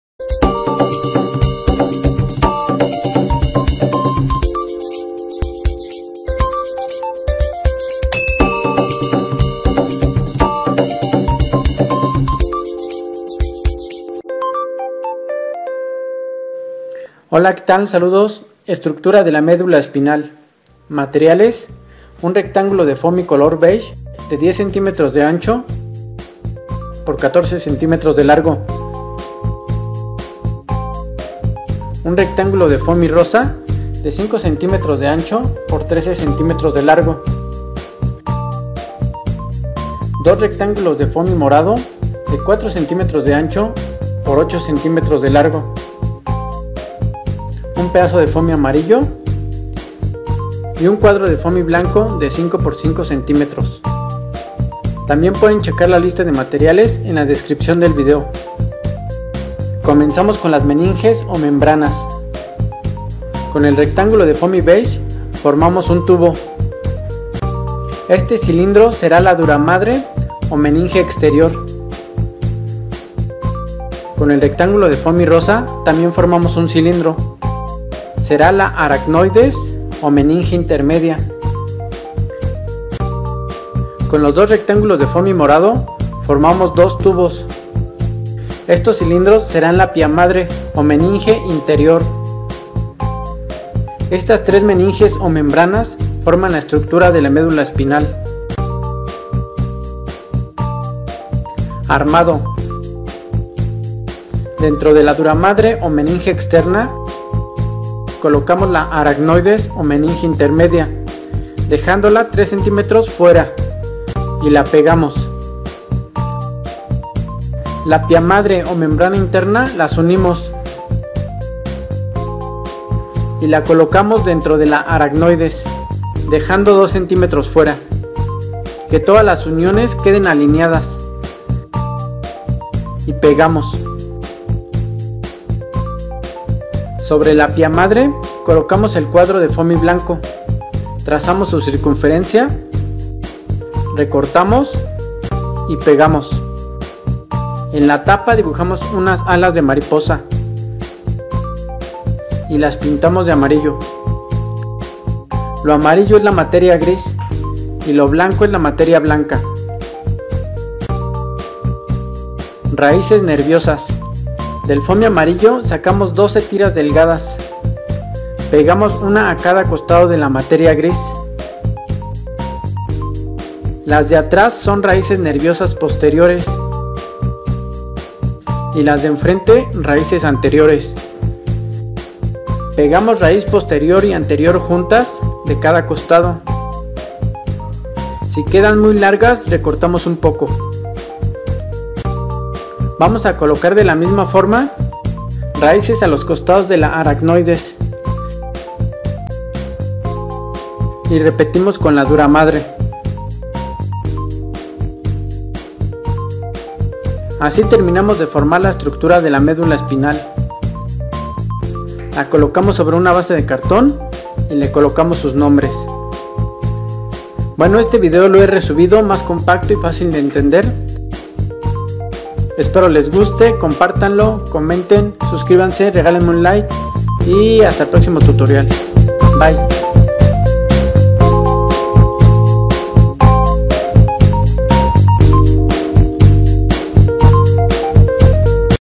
Médula Espinal maqueta (Model Spinal Cord)
MATERIALES:

- Rectángulo de fomi color beige 10 X 14 cm
- Rectángulo de fomi rosa 5 X 13 cm
- Dos rectángulos de fomi morado 4 X 8 cm
- Fomi amarillo
- Cuadrado de fomi blanco 5 X 5 cm

MATERIALS:

- Rectangle of beige fomi 10 X 14 cm
- Rectangle of fomi pink 5 X 13 cm
- Two rectangles of purple fomi 4 X 8 cm
- Fomi yellow
- Square of fomi white 5 X 5 cm